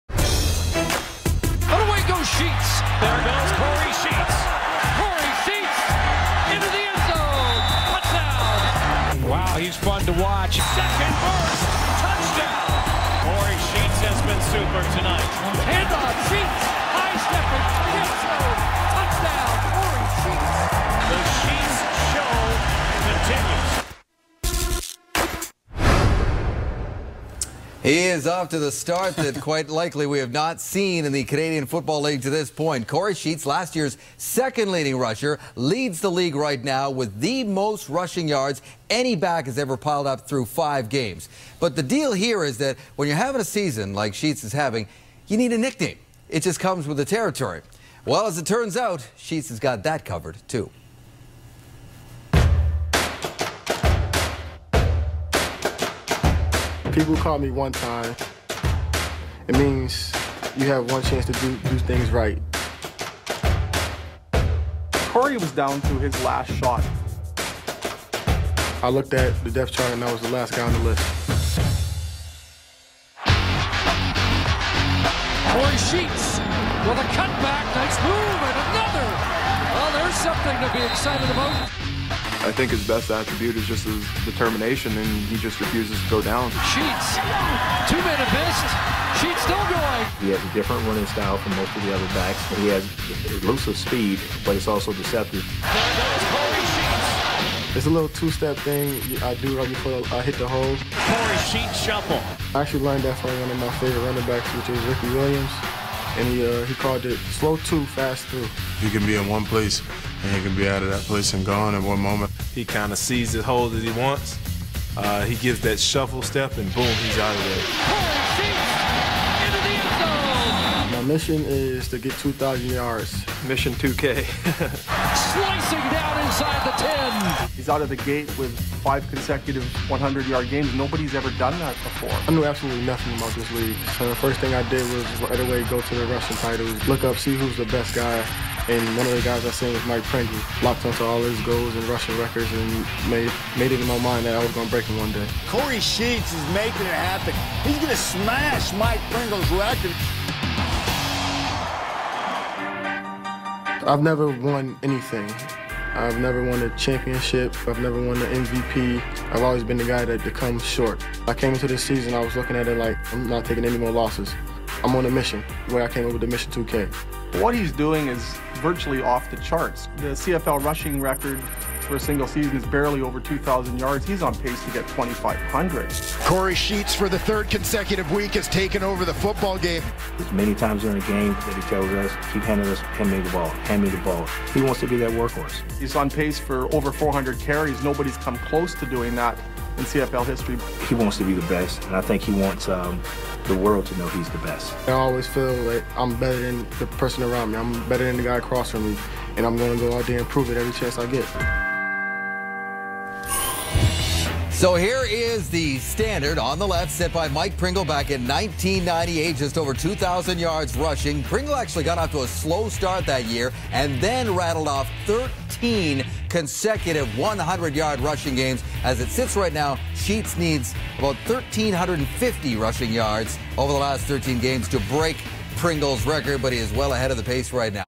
Kory Sheets TSN Mission2k Saskatchewan Roughriders CFL feature
TSN profiles Kory Sheet's